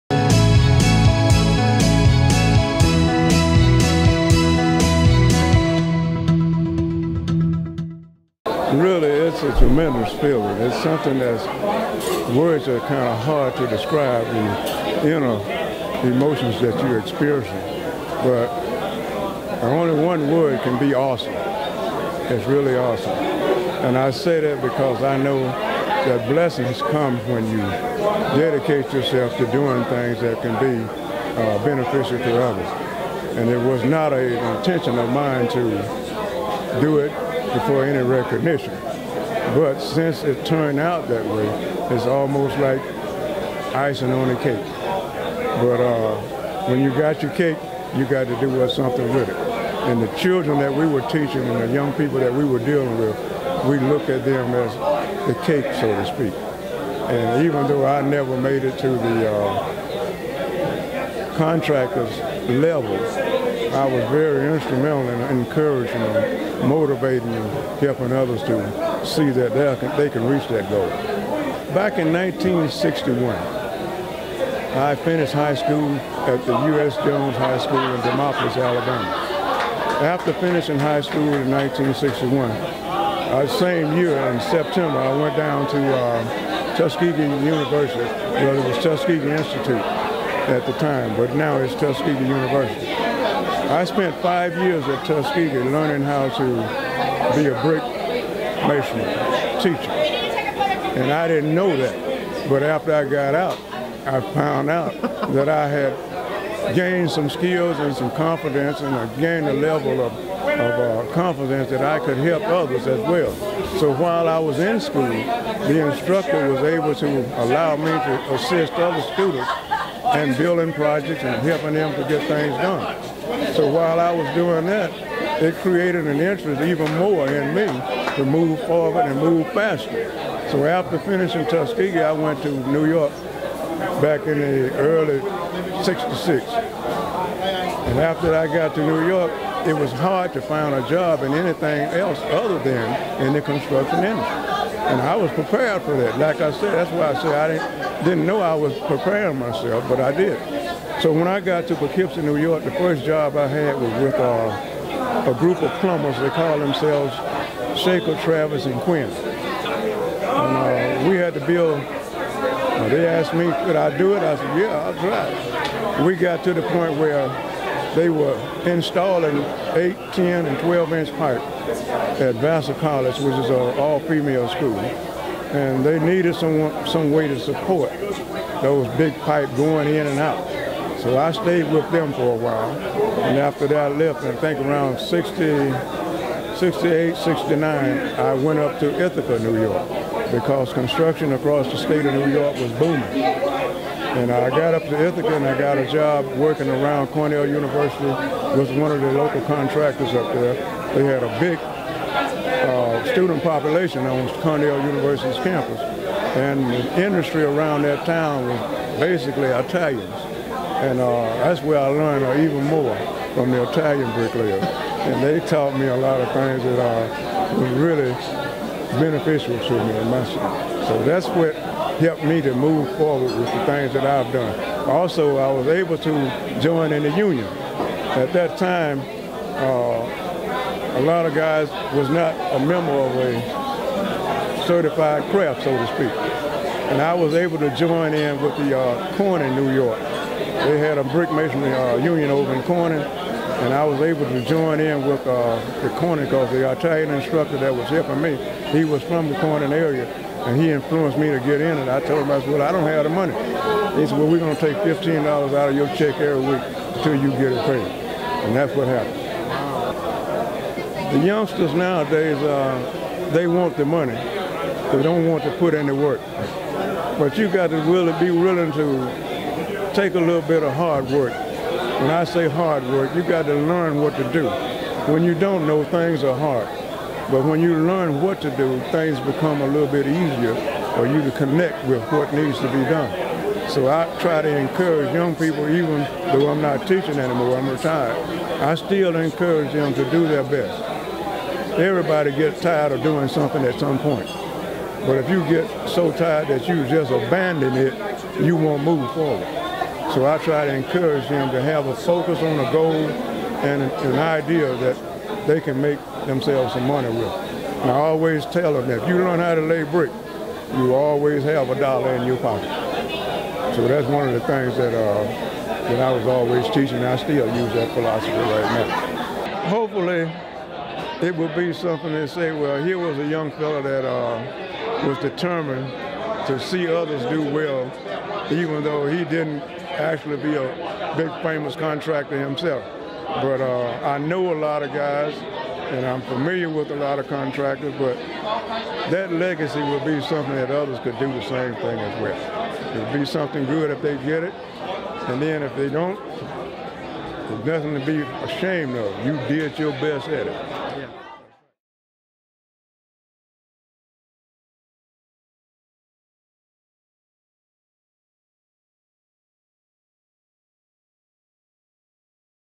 Interview with Milton H. “Milt” Young, II
Milton H. “Milt” Young, II discusses his induction into the Masonry Hall of Fame, his career in the masonry industry, and his legacy.